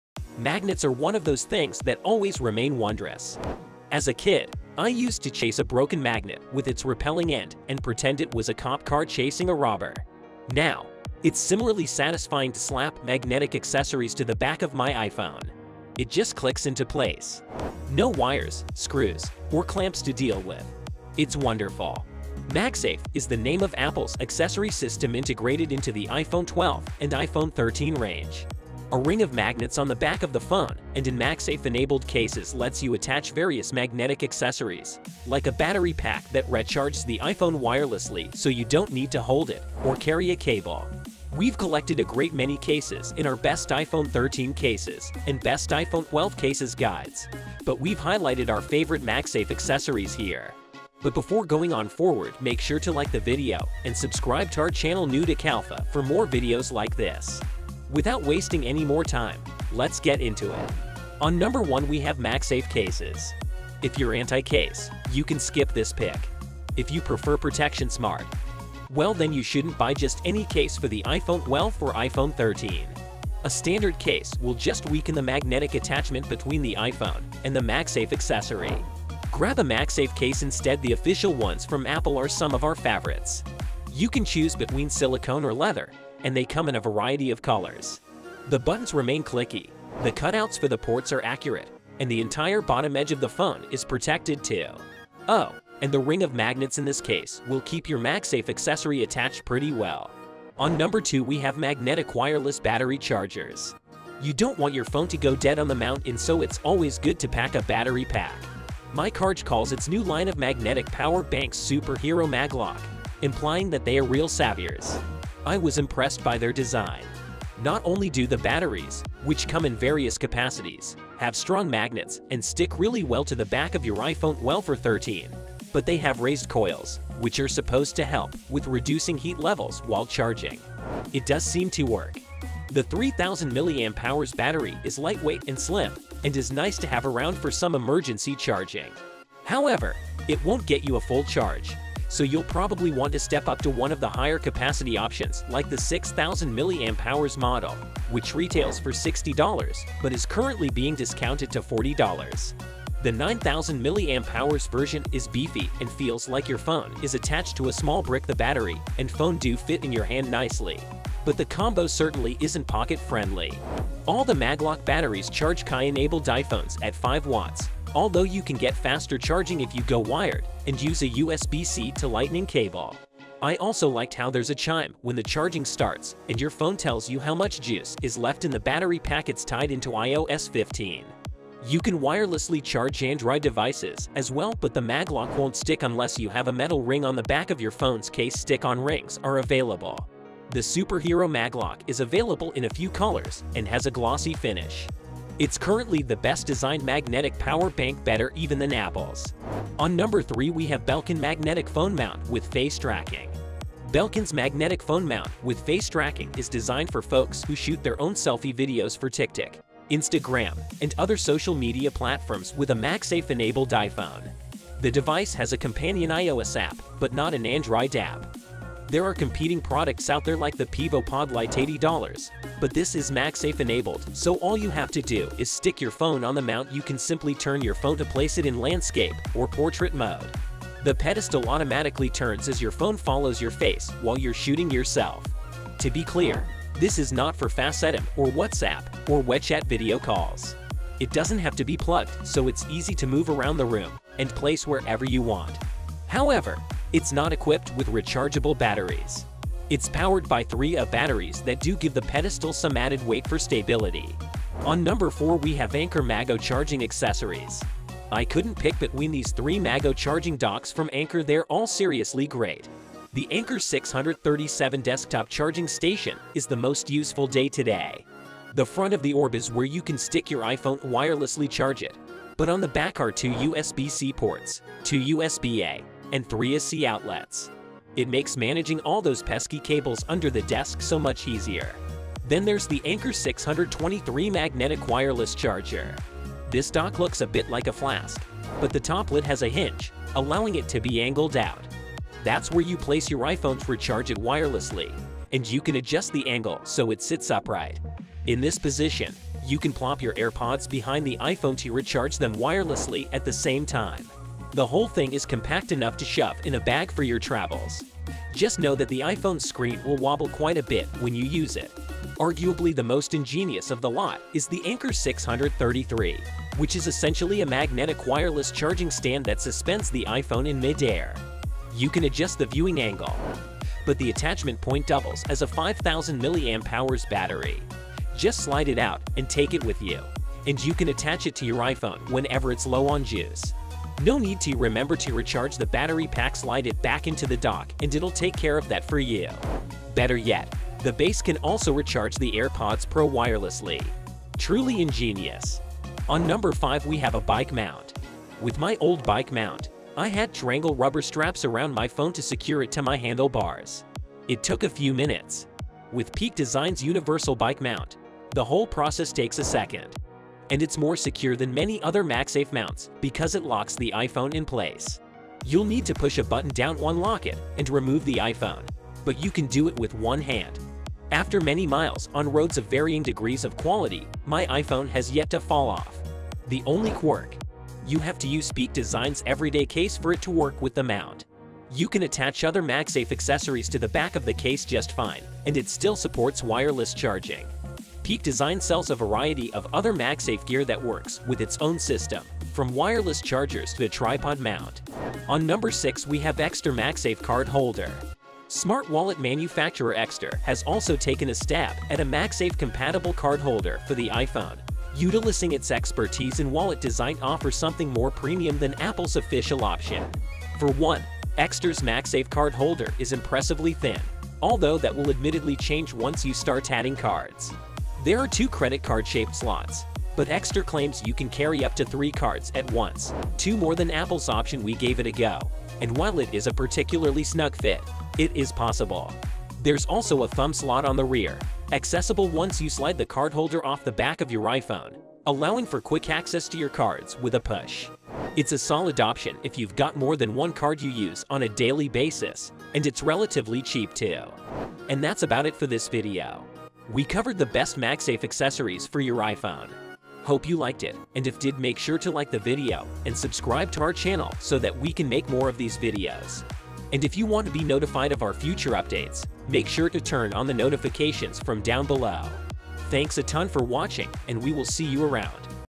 Best Magsafe Accessories for iPhones and iPads
The iPhone 13 and iPhone 12 (in all their mini, Pro and Pro Max models) are unique because they are compatible with Apple's MagSafe charging system. They have a magnet built directly into them so that, when paired with a MagSafe charger, the iPhone magnetically snaps in and charges. It eliminates the need to find that perfect alignment that most wireless chargers still require. More fun is that MagSafe opens up the door for whole new ecosystem of accessories that work with the new iPhone models. Apple has already released a number of MagSafe cases, chargers and even a minimalist wallet. But more and more third-party makers are coming out with MagSafe accessories of their own. We've rounded up a whole host of them, for iPhone 13 and iPhone 12 alike, here.
Also, don’t forget to keep the bell on a ring to notify you on our future uploads.